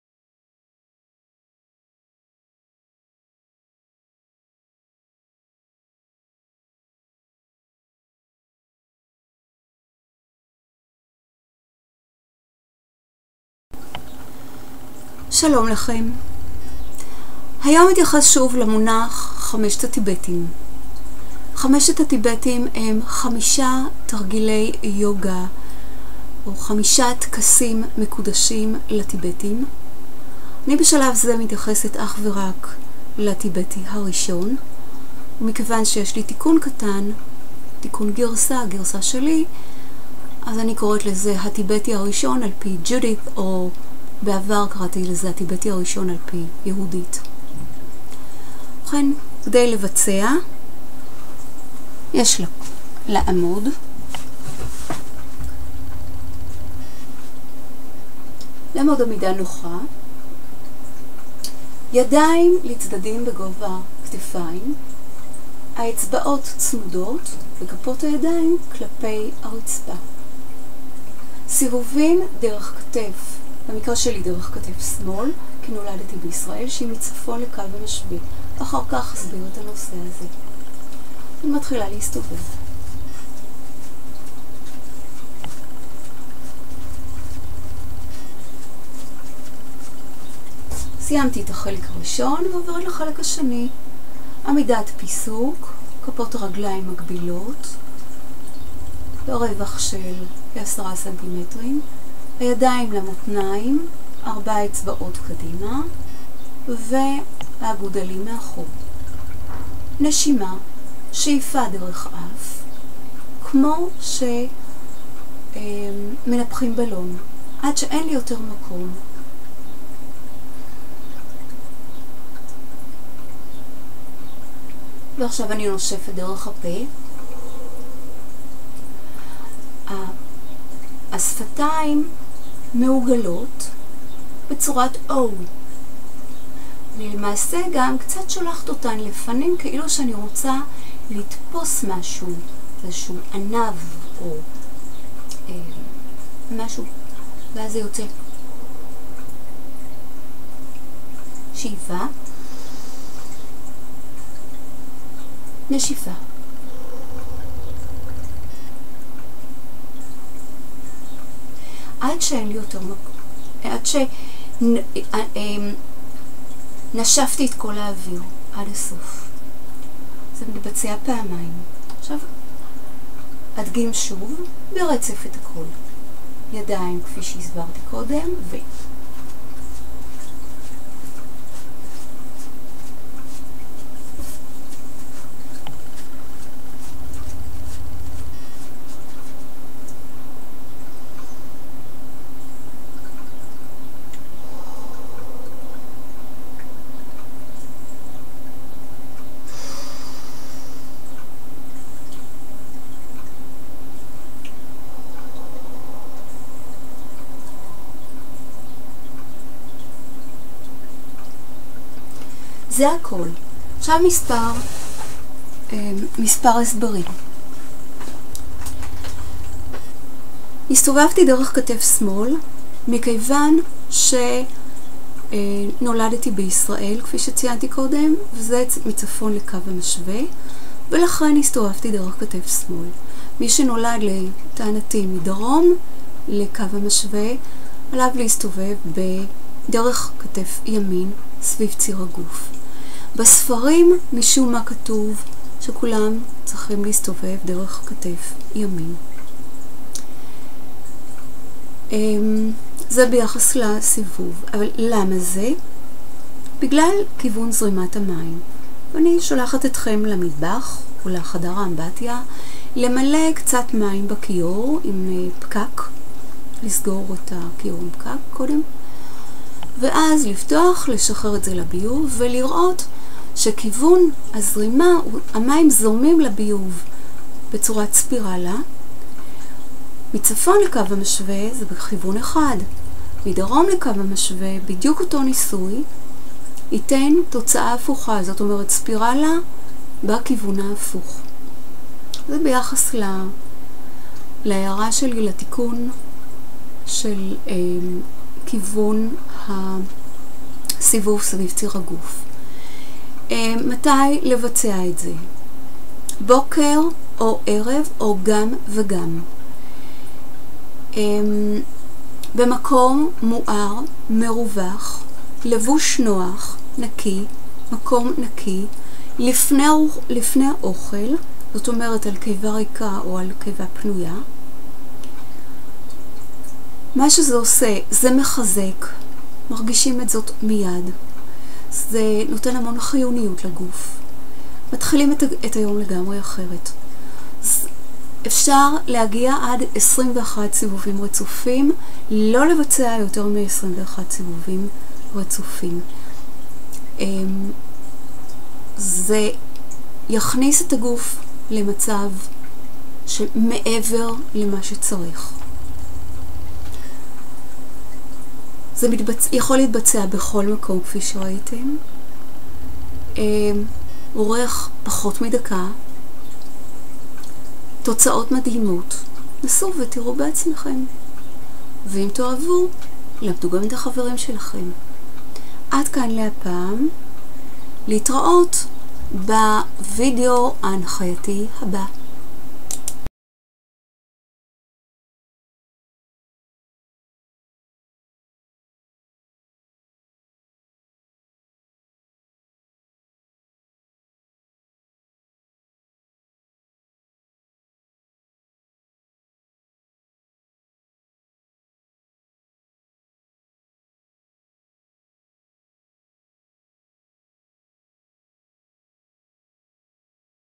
הסודות של ג'ודית לתיחזוק עצמי - הטיבטי ה-1 עפ"י ג'ודית
הקליפ הזה מדגים בהנחיה מלאה את הטיבטי הראשון על פי ג'ודית. זהו קליפ שלישי של ג'ודית בנושא הזה המקיף פחות או יותר את כל הנושא הנקודתי הזה. ראוי רצוי שיתבצע במקום מואר, מאוורר היטב, מרווח, נקי, בלבוש נוח ונקי
פרטים וטיפים רב תחומיים נוספים תמצאו בדף האוהדים בפיסבוק